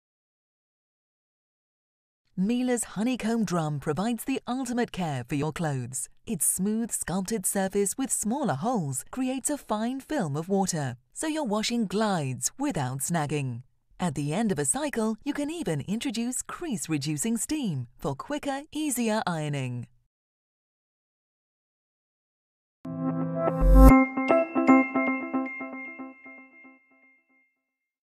Miele Washing Machine HoneyComb Drum | ao.com
Check out our short video explaining the Honeycomb drum feature of Miele’s washing machines. These special drums create a thin layer of water so your clothes glide without snagging on anything. You can even add in some steam so your clothes are easy to iron.